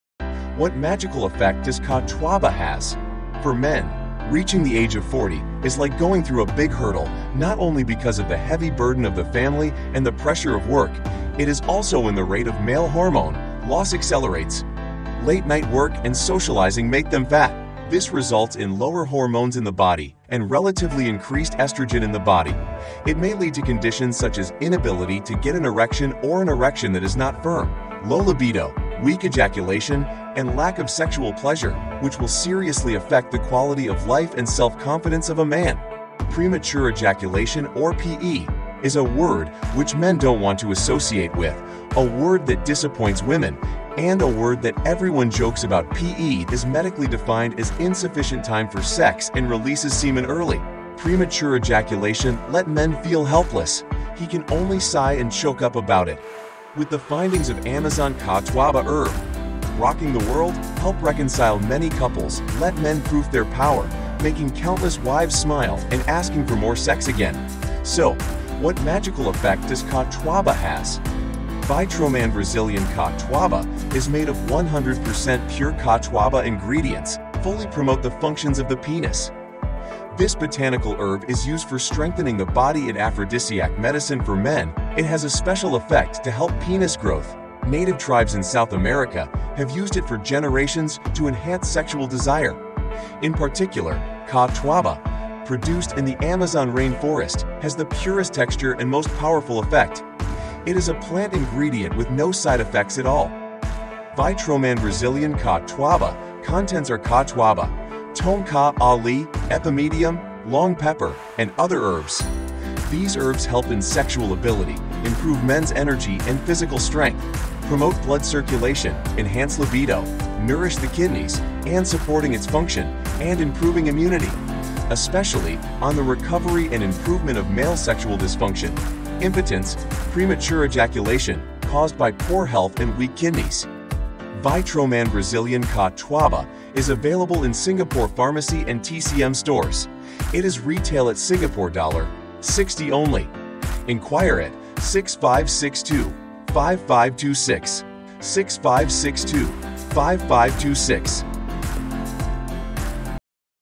What magical effect does This botanical herb is used for strengthening the body ?
What magical effect does Catuaba have? For men, reaching the age of 40 is like going through a big hurdle, not only because of the heavy burden of the family and the pressure of work, it is also when the rate of male hormone loss accelerates. Late night work and socializing make them fat. This results in lower hormones in the body and relatively increased estrogen in the body.  It may lead to conditions such as inability to get an erection or an erection that is not firm, low libido, weak ejaculation, and lack of sexual pleasure, which will seriously affect the quality of life and self-confidence of a man.

Premature ejaculation or P.E. is a word which men don’t want to associate with, a word that disappoints women, and a word that everyone jokes about. P.E. is medically defined as insufficient time for sex and releases semen early.

Premature ejaculation lets men feel helpless, he can only sigh and choke up about it. The findings of Amazon Catuaba herb, rocking the world, help reconcile many couples, let men prove their power, make countless wives smile, and ask for more sex again.

So?    What magical effect does Catuaba have?

Vitroman Brazilian Catuaba is made of 100% pure Catuaba ingredients, fully promoting the functions of the penis.

This botanical herb is used for strengthening the body and aphrodisiac medicine for men, it has a special effect to help penis growth. Native tribes in South America have used it for generations to enhance sexual desire. In particular, Catuaba produced in the Amazon rainforest has the purest texture and most powerful effect. It is a plant ingredient with no side effects at all.

Vitroman Brazilian Catuaba contents are Catuaba, Tongkat Ali, Epimedium, Long Pepper, and other herbs. These herbs help in sexual ability, and improve men's energy and physical strength. Promote blood circulation, enhance libido, nourish the kidneys and support their function, and improve immunity. Especially, on the recovery and improvement of male sexual dysfunction (impotence, premature ejaculation) caused by poor health and weak kidneys.

Vitroman Brazilian Catuaba is available in Singapore pharmacies and TCM stores. It is retail at Singapore Dollar 60 only. Enquire at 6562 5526.